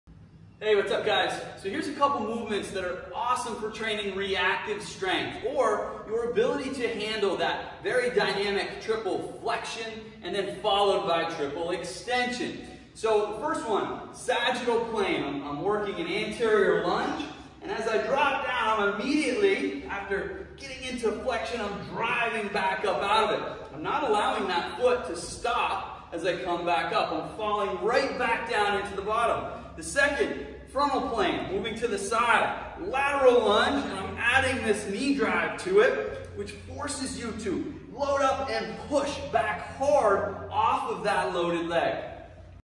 Band Resisted Reactive Forward and Lateral Lunge
These are a great way to help develop reactive strength in the lower body.